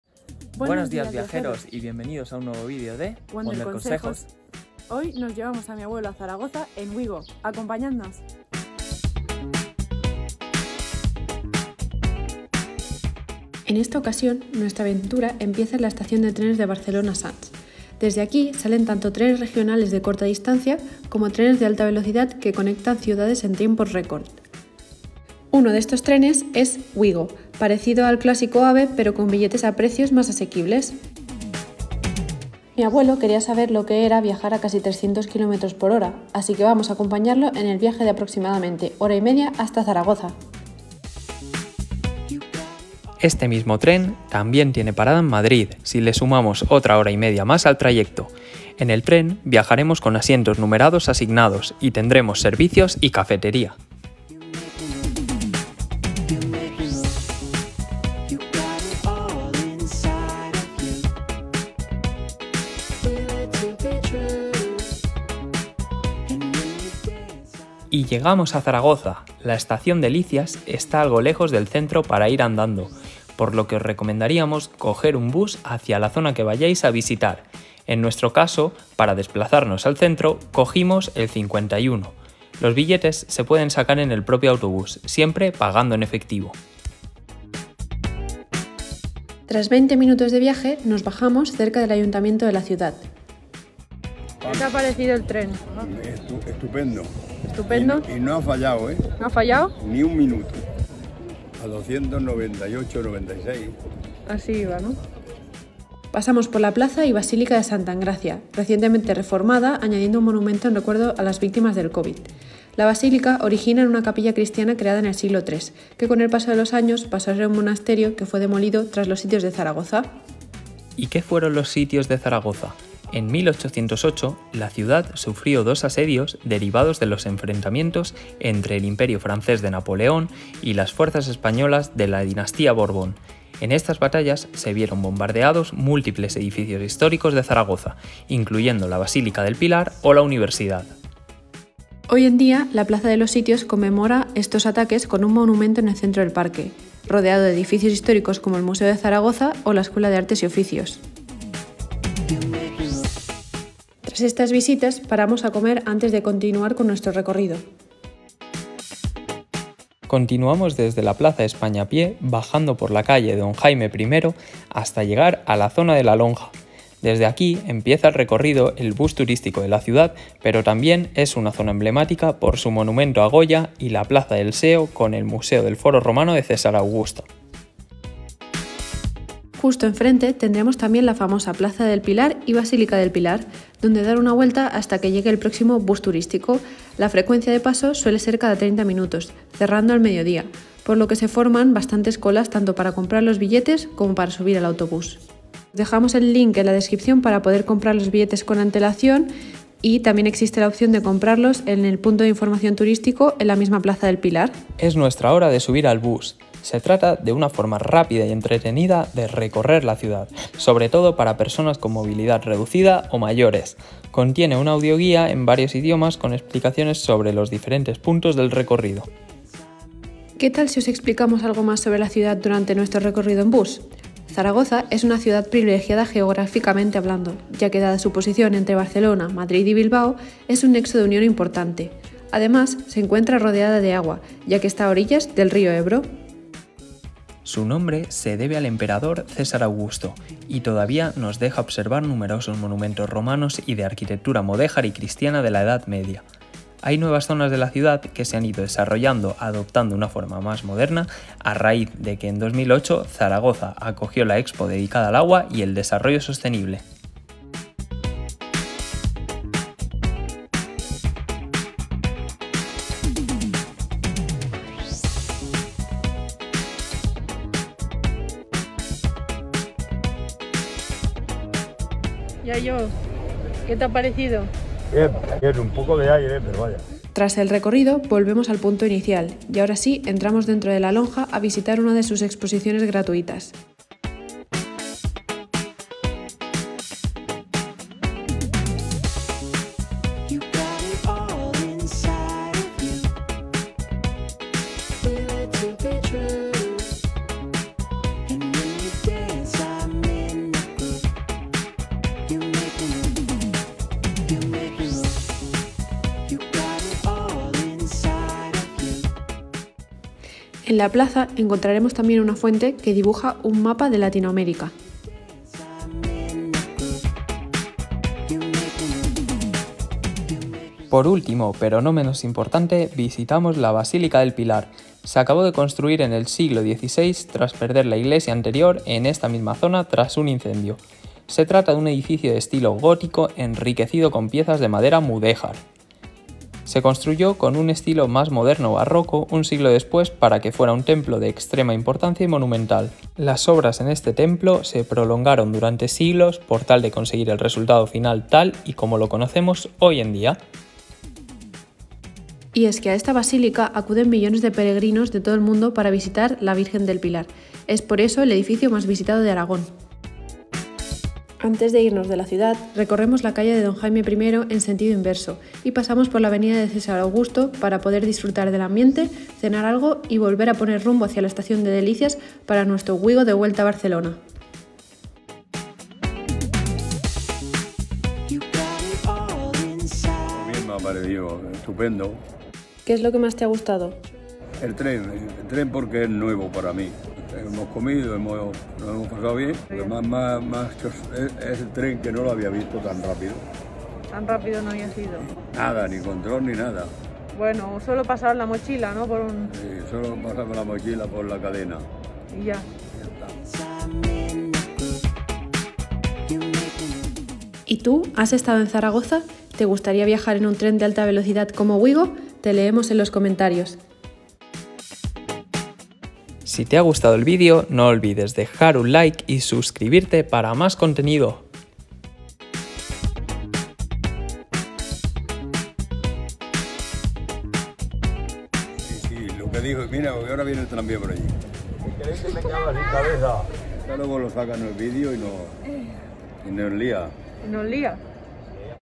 Llevamos a mi abuelo a Zaragoza en OUIGO | Un día en Zaragoza
¡Hola! ¡Somos Alba y Joaquín y nos encanta viajar!

En el vídeo de hoy acompañamos a mi abuelo a visitar Zaragoza en el tren de alta velocidad Ouigo. ¿Nos acompañais vosotros también?

¡Bienvenidos al mundo wonderconsejos!